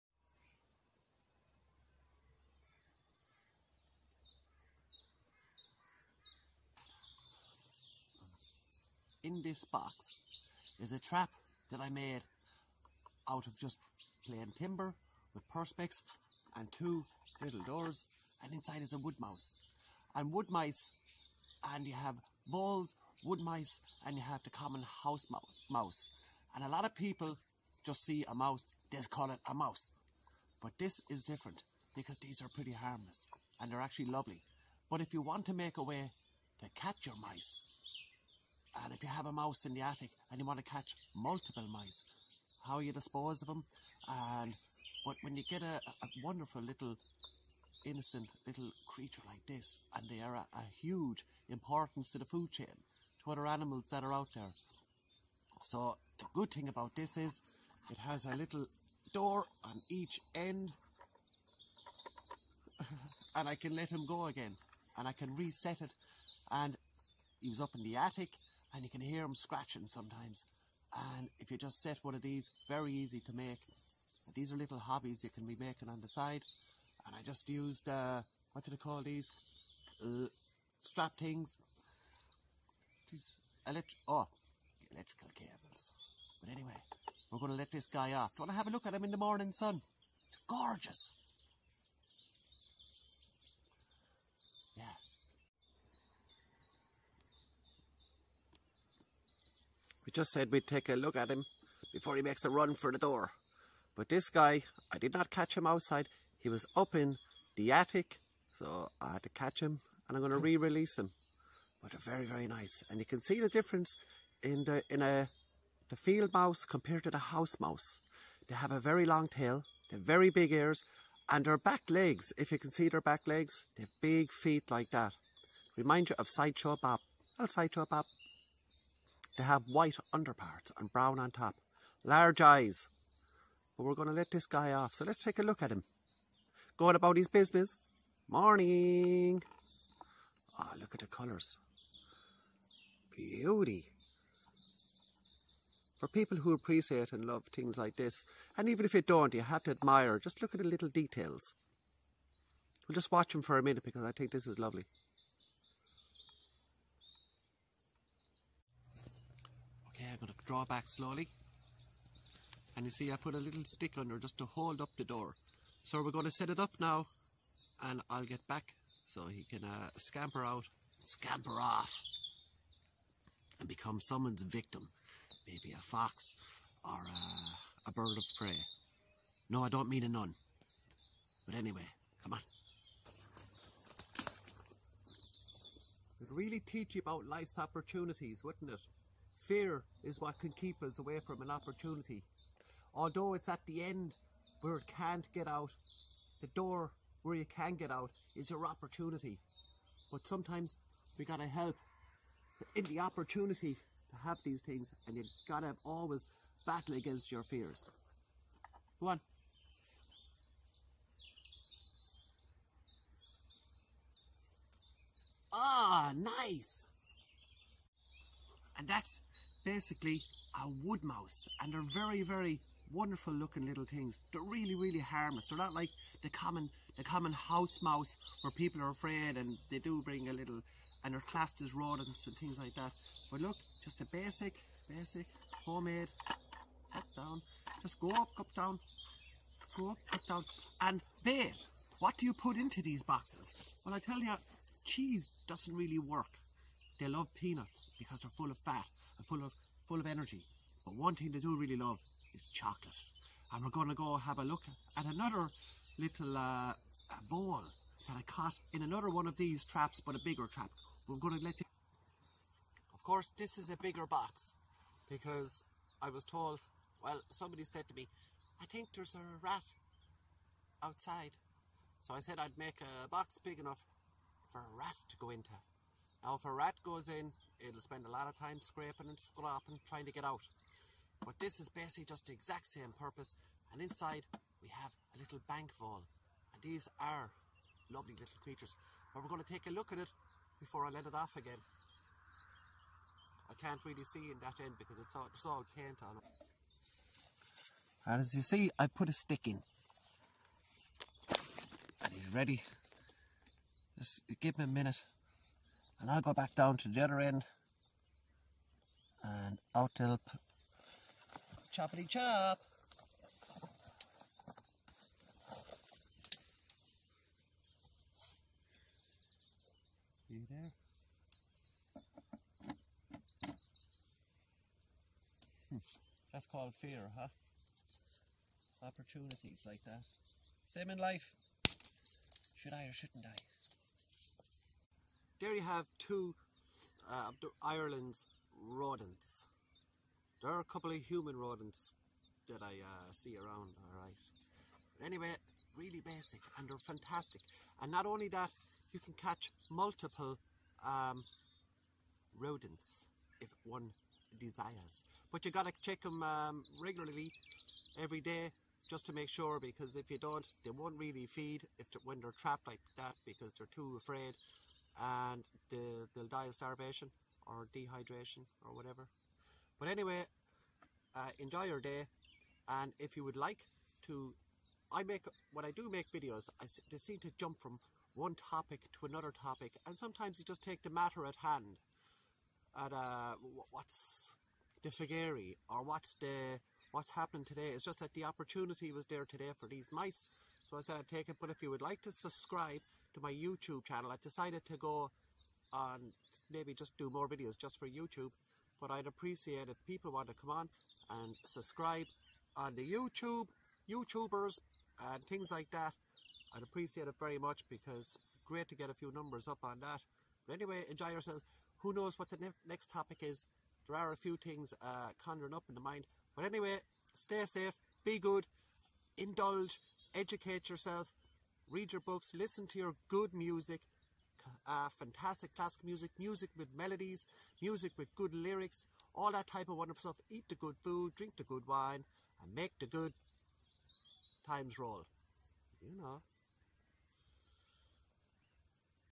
Irish wildlife
A look at two of Ireland's small rodents.